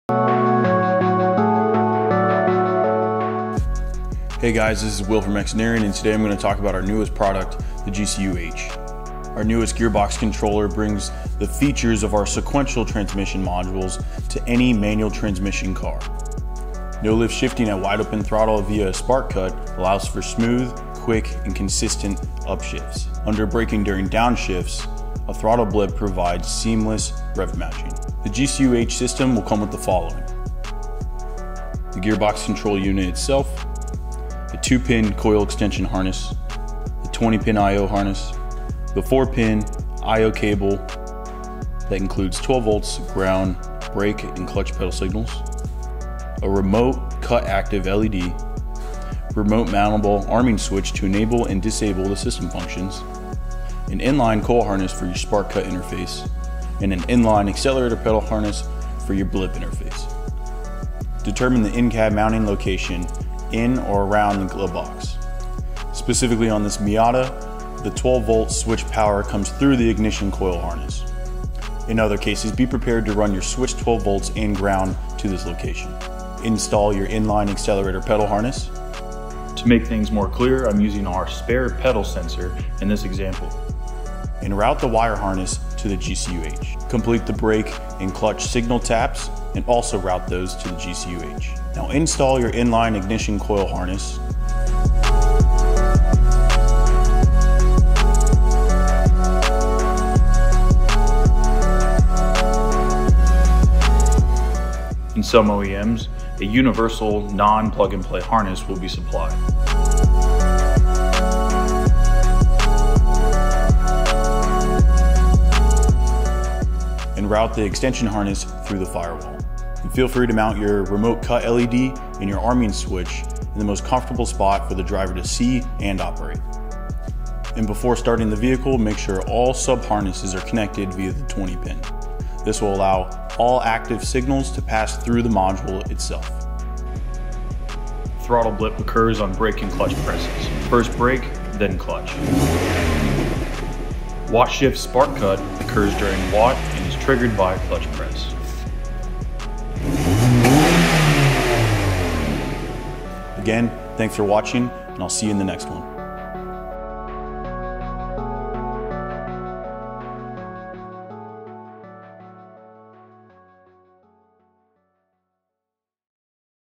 Flat Foot Shifting and Downshift Blip for H-Pattern Manual! - GCU-H: WOT Shift Rev Match Module
Our newest Gearbox Control Unit allows you to Wide Open Throttle (WOT) or Flat Foot Shift (FFS) AND will rev match your downshifts!!

In this video we discuss what the module can do and how to install it on our example Miata.

By monitoring clutch presses and using a set accelerator pedal percentage, we can cut spark to the engine allowing you to stay on the accelerator during an upshift. The module will then look for the clutch to be released to introduce spark to the engine.
The spark cut period is variable (closed loop) based on clutch press IN/OUT
After clutch out spark is ramped back from the cut % to full power linearly over the Torque
Recovery period. This softens the reintroduction of power thru the drivetrain to minimize shock
and improve traction. But it can also be set to zero to the quickest application of power.
Minimum Throttle Pedal % input must be met for Spark Cut to be active.
NOTE: If the clutch IN/OUT period exceeds the timeout period (example: 300ms) the spark
energy is returned to the engine.
Some OEMs, including Mazda, GM and Viper will come with a plug and play ignition harness to cut spark. Other OEMs will be equipped with a universal harness to cut spark.

Downshift rev matching is included for consistent braking during downshifts, no more unsettling the car trying to heel-toe.
A throttle blip (to a programmable % and duration) will occur when the brake
pedal is pressed followed by a clutch press. NOTE: If the clutch is pressed first
and then the brake, no blip will occur.

Programmable parameters:
- WOT pedal (%) - Min accelerator pedal % to be consider WOT
- Spark Cut (%) - Depth of spark cut
- Spark Cut Duration Timeout (ms)
- Spark Cut Torque Recovery (ms)
- Blip Target (%)
- Blip Duration (ms) - Duration of throttle blip after brake & clutch press
- Brake Pedal press % (if applicable C6 Corvette & 5th Gen Camaro, etc)
- Clutch Pedal press % (if applicable ex 08-13 Corvette & 5th Gen Camaro)

Our Core Values:
S.W.E.A.T
Service First
Whatever It Takes
Excellence Is Our Standard
Always Do The Right Thing
Team is greater than One